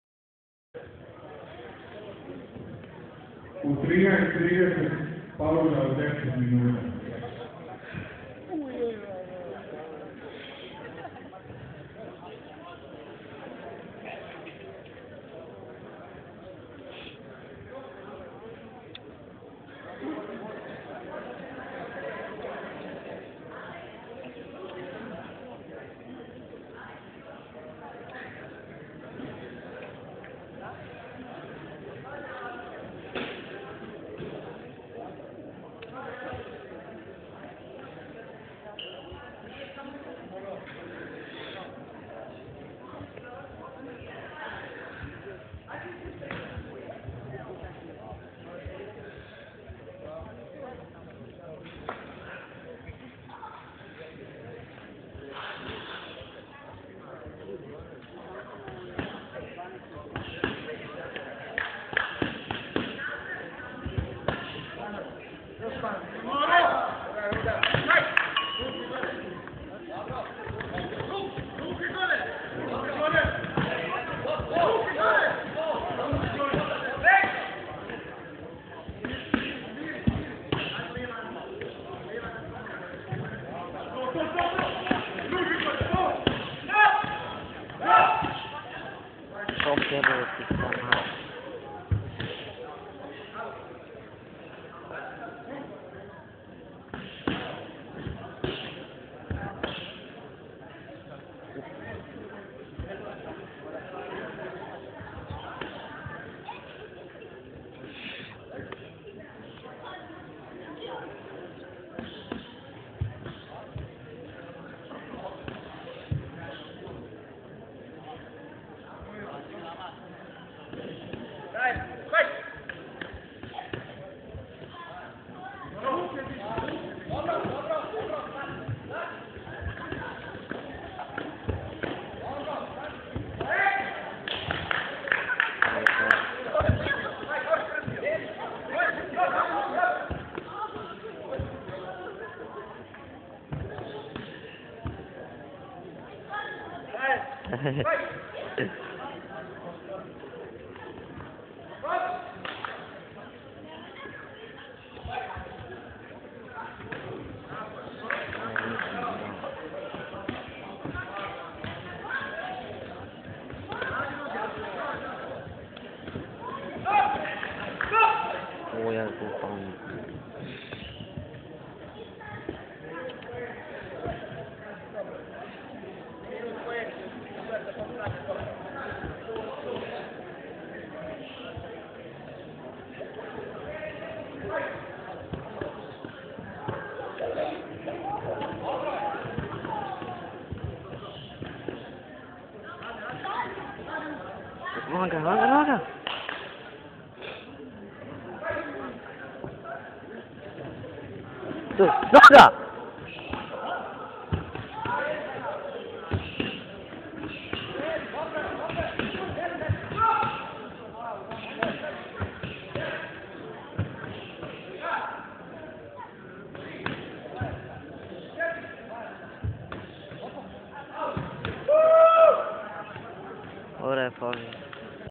Danijel Popović VS Mirnes Selimi (Finalni meč)
Kick Box Open Slavonski Brod
Pravila: Low Kick -75kg,
Finalni meč.
Danijel Popović Crni šorc,
Mirnes Selimi Crveni šorc,

Pobjednik meča: Danijel Popović.
Trener: Husić Sanjin
U ćošku: Puzić Azra, Midhat Mustafagić
Kamerman: Mustafa Mašetović